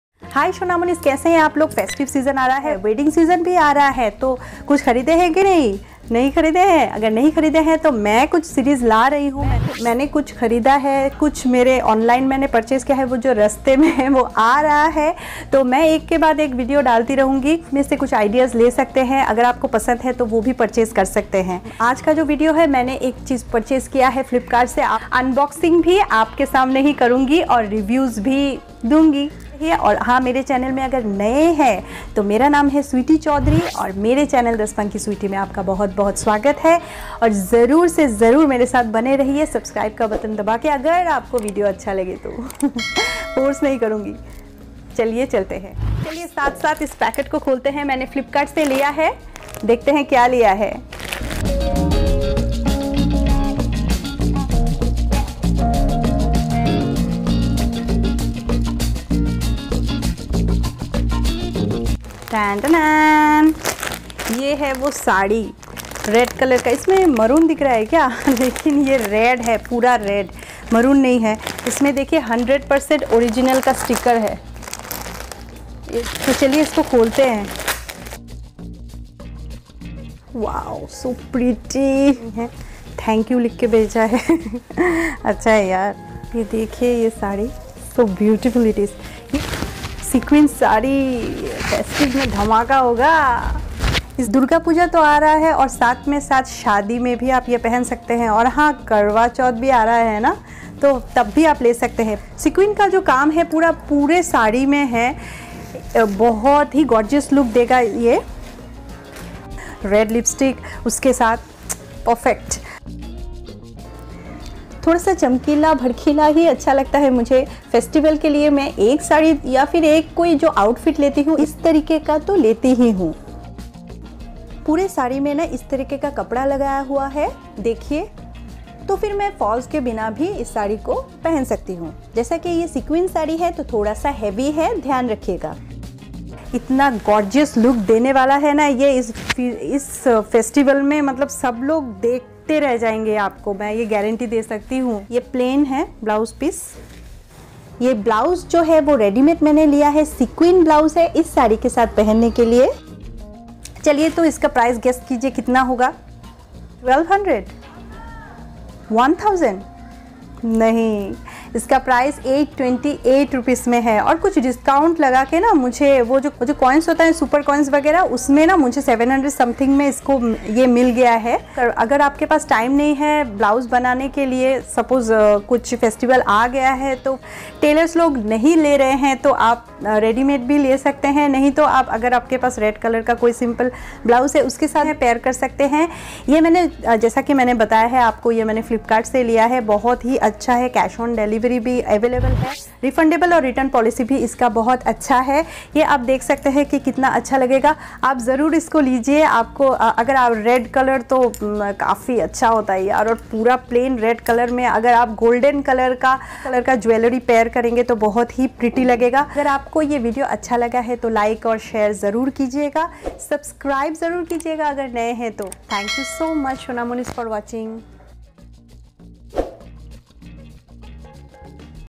DURGA PUJA Special Saree II Flipkart Festive Wear 2022 The SpunkySweety
Flipkart Festive Wear 2022 II Durga Puja Special Saree II The SpunkySweety

Hi Shonamonis 🙋😘

This type of outfit I always prefer to buy at least one for any big FESTIVAL or PARTY or for WEDDING SEASON. I just love it 🥰. So, aap ko ye Lena hai k nahi ye video dekke aap ko pata chal jayega kyu ki UNBOXING  ke saath FULL REVIEW bhi Diya hai.. Go grab it 👍😉.

Link of the product:

Video Highlights:
Saree for Durga Puja
Durga Puja special Saree
Flipkart saree below 1000
Durga Puja saree look 2022